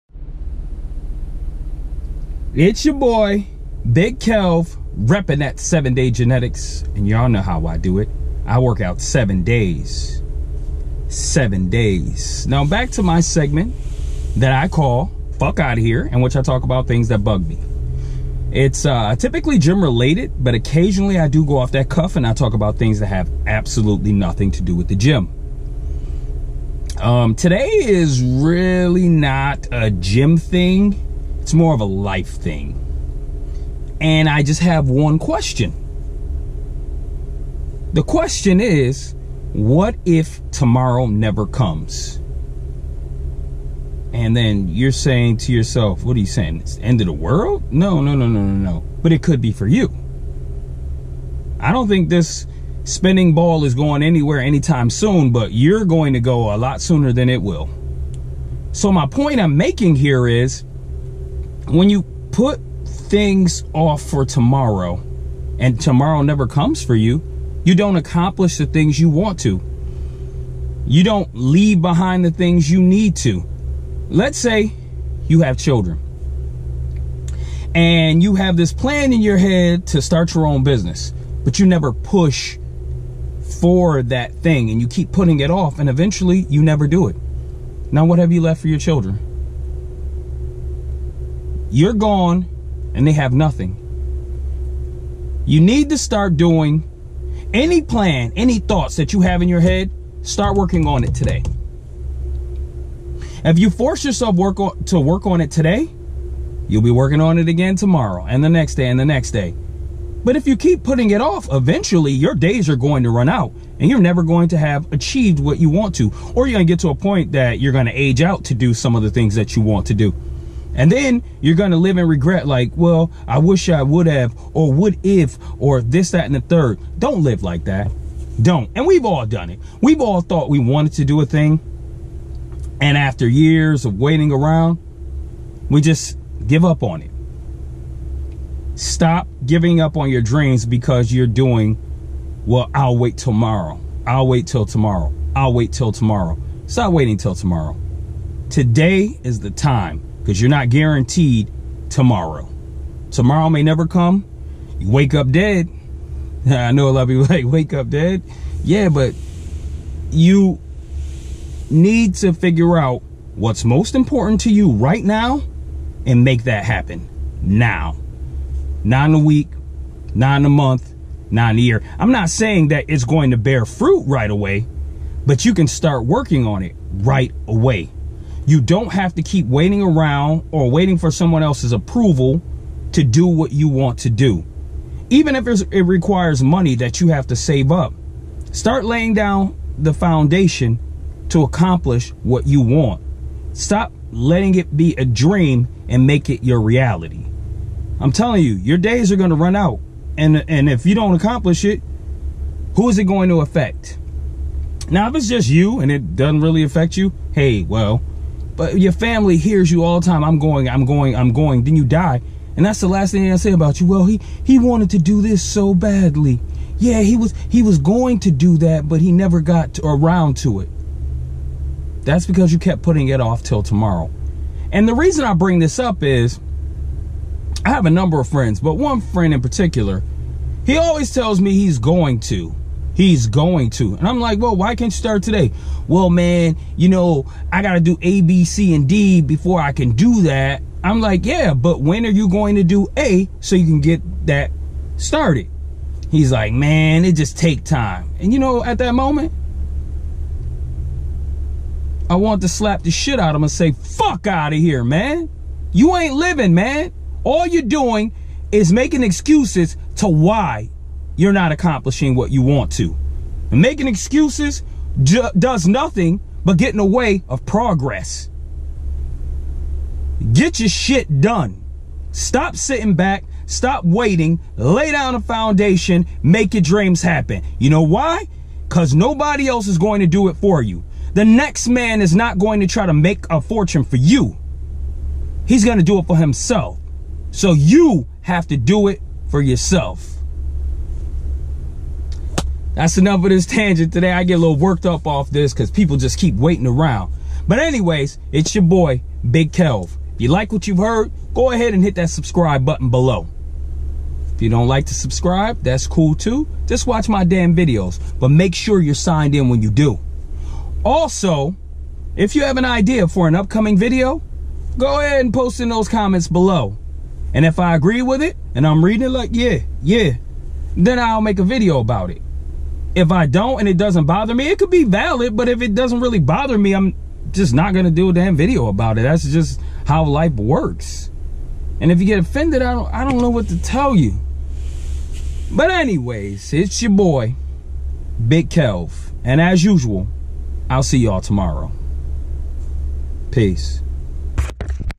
F*ck Outta Here Rant - Episode 98 - Stop putting things off for tomorrow!
Today is the first day of the rest of your life!